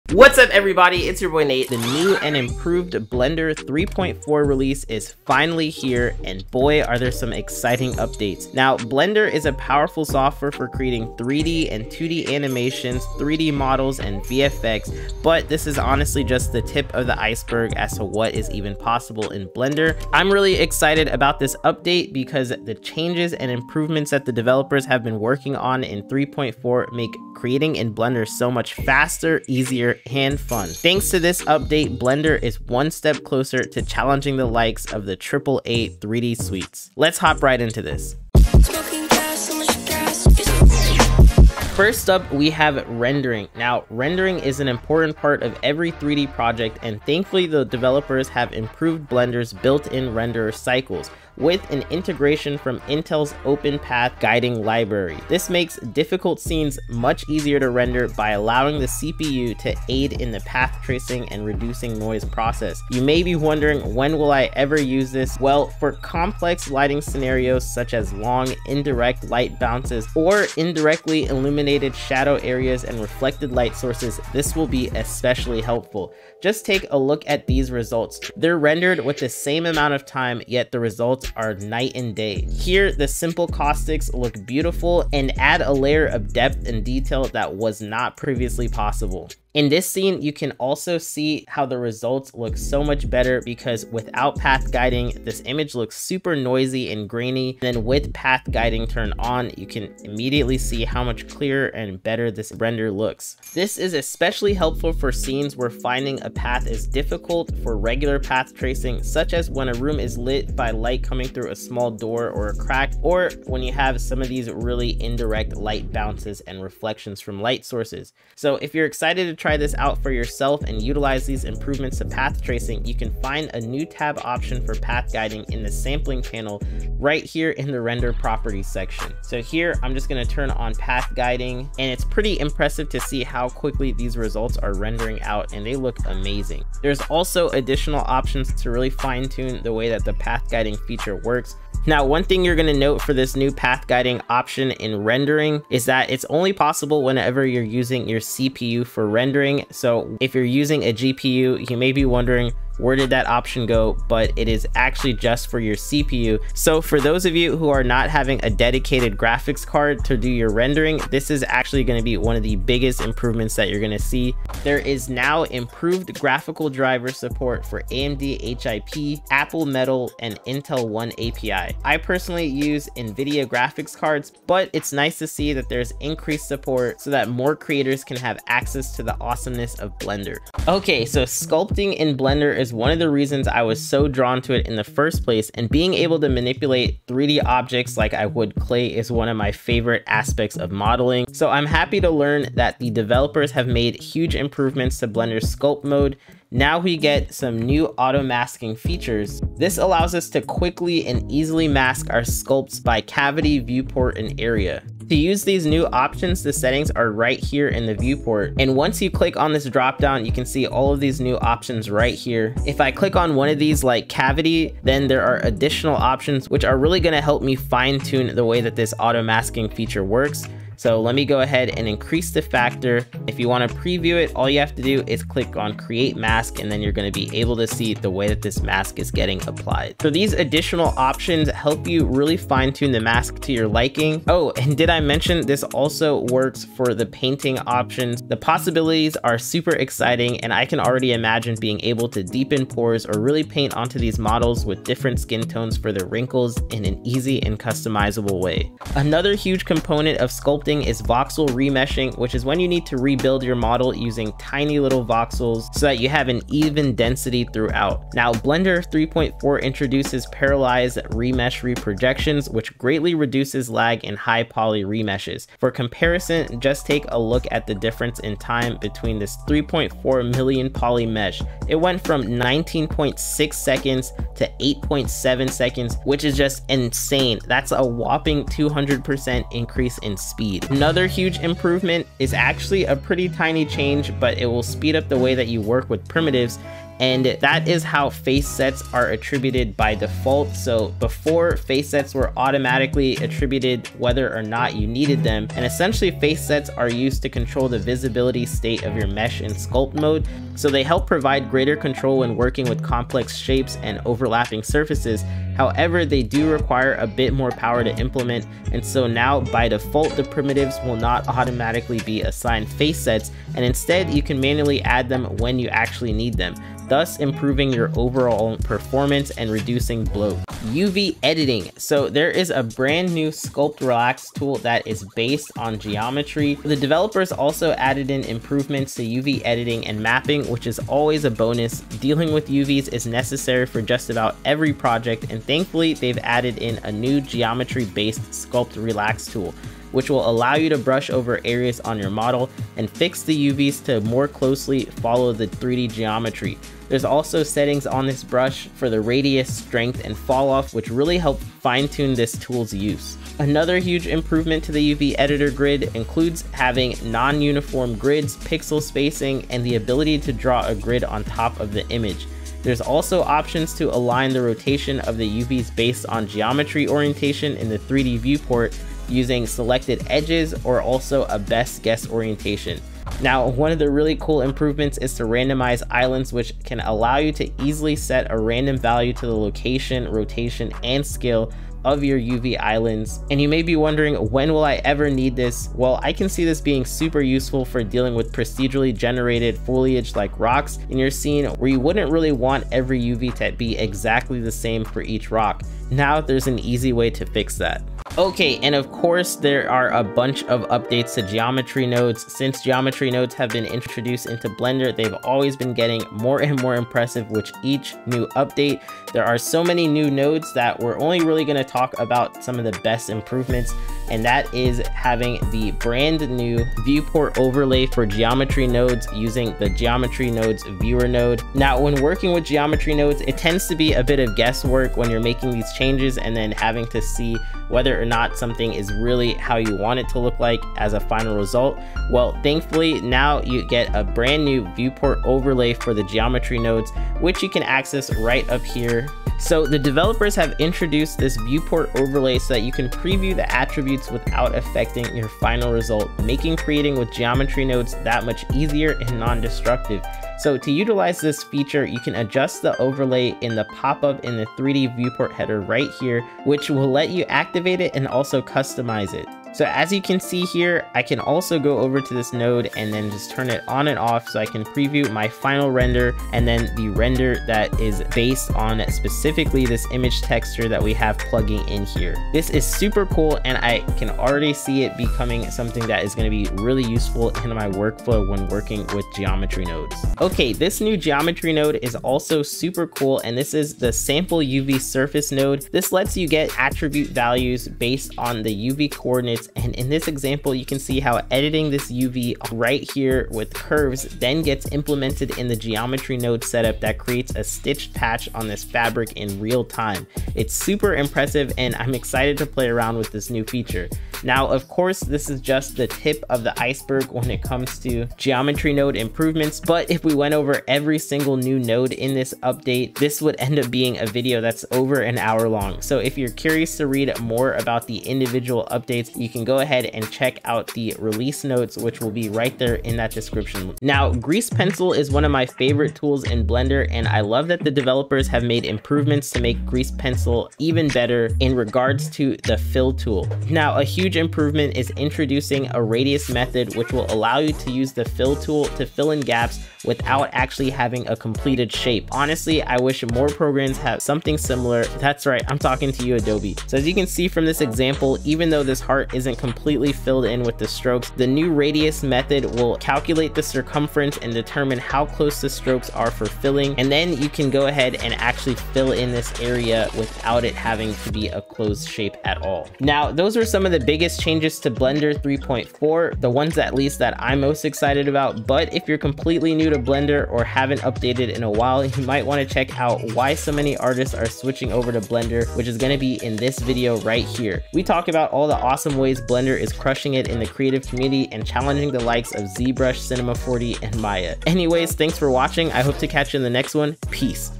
Blender 3.4 is Crazy Awesome!
Blender just got a huge upgrade! There are massive improvements to rendering, animation, sculpting, geometry nodes, and so much more. With so many updates and features being added, I had to share some of the newest and most exciting aspects with you all. That way you can make an informed decision as to whether or not updating to Blender 3.4 is worth.

My Favorite Motion Graphics Assets, Plugins, Templates, and MORE!!